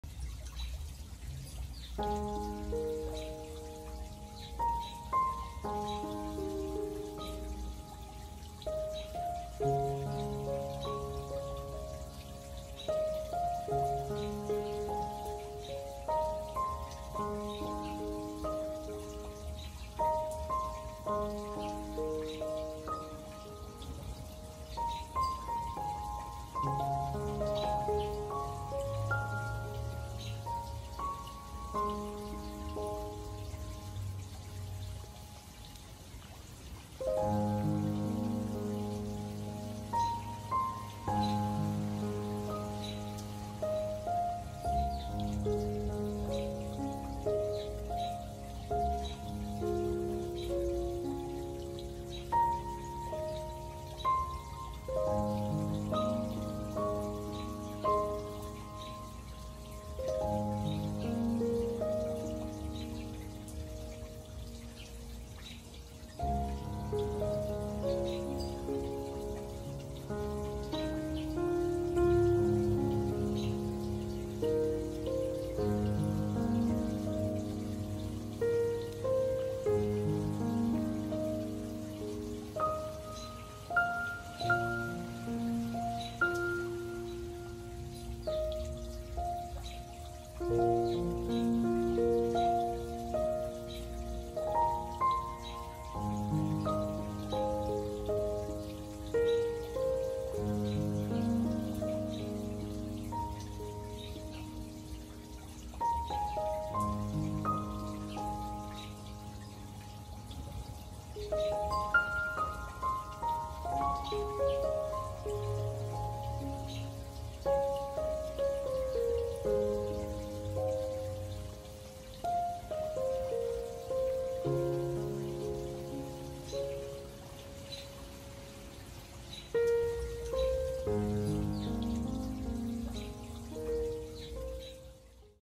Butterfly / Relaxing Nature Video / Bird Sounds at the Water Fountain
Enjoy this relaxing nature video of beautiful butterfly enjoying wild flowers with relaxing music & bird sounds at the water fountain. This video was shot with iPhone 11 pro.